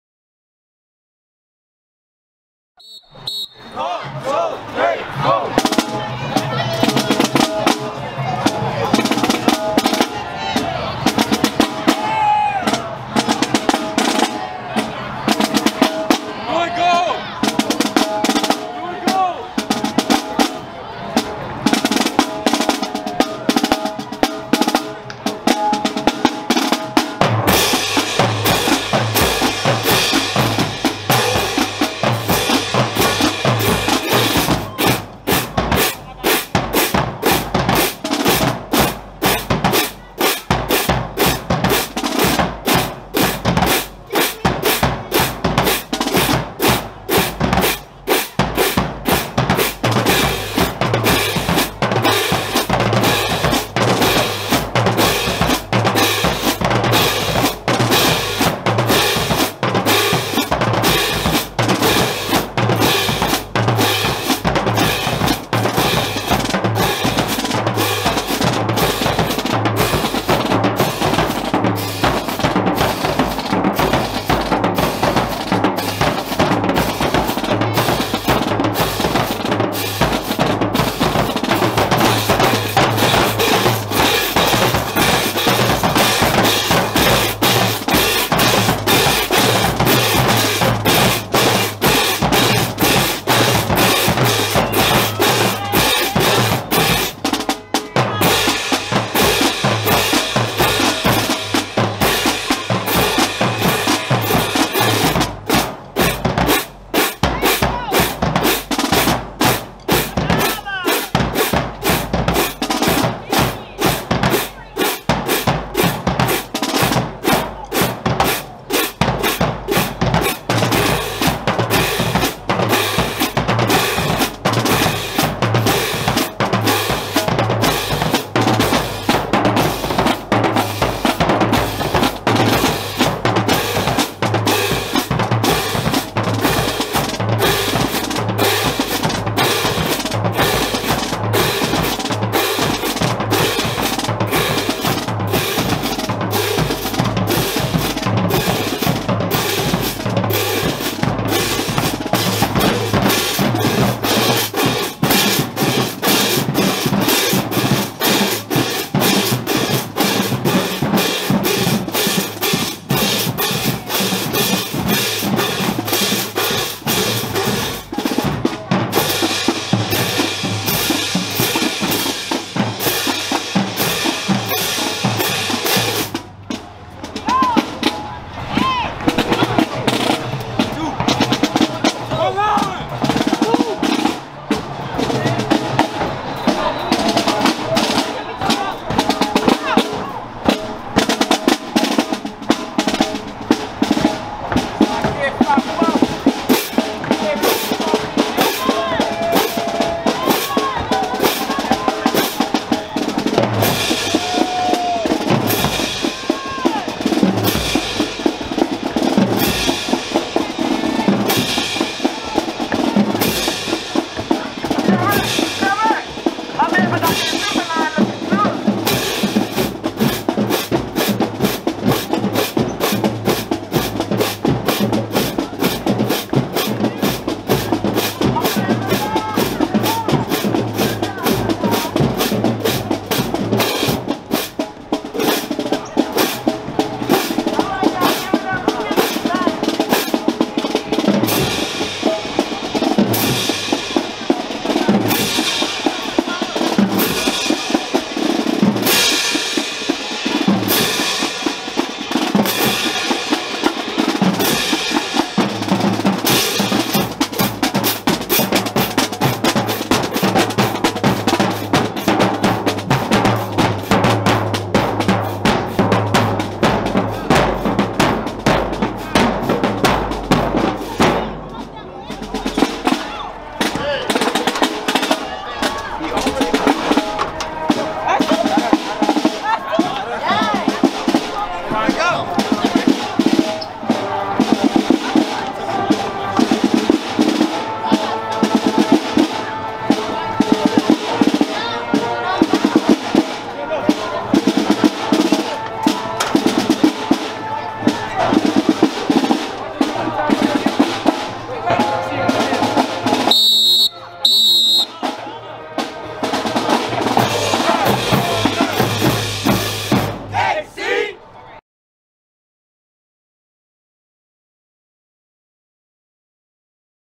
John F Kennedy marching band 2022 Entering Stadium @ Reed game.
Official Video highlight clip featuring the John F Kennedy marching band coming into the stadium before the start of game. Highlighting All marching bands this season.